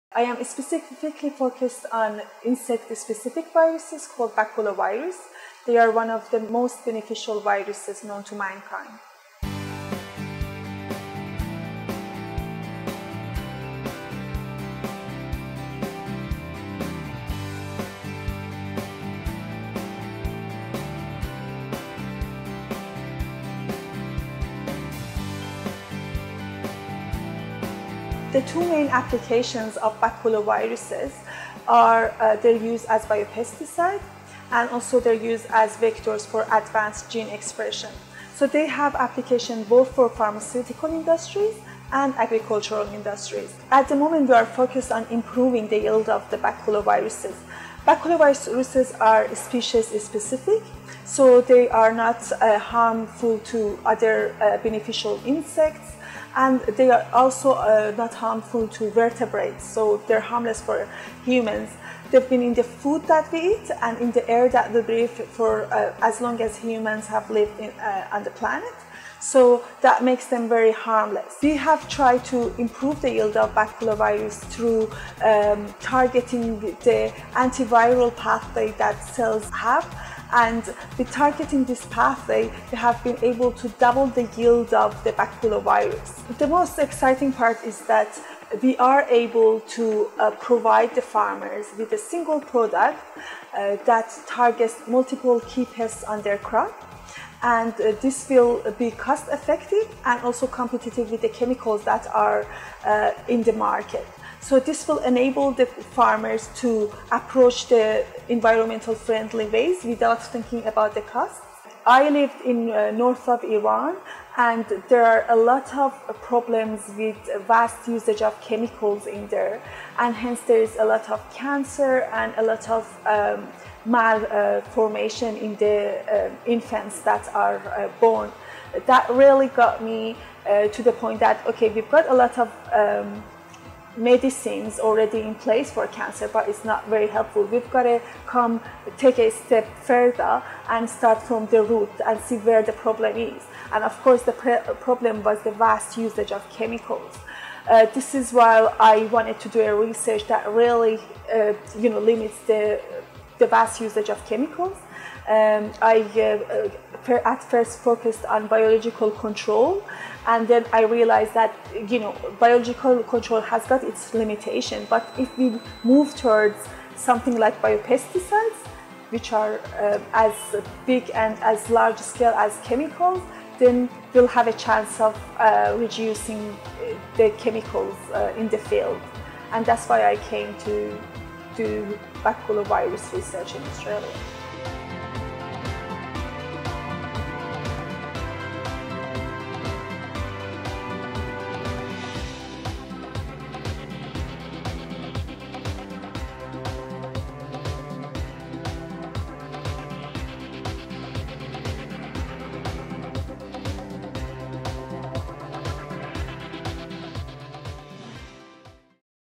QLD Fresh Science 2016 – Leila Matindoost
P35-Enhanced insect cell line increases the yield of baculovirus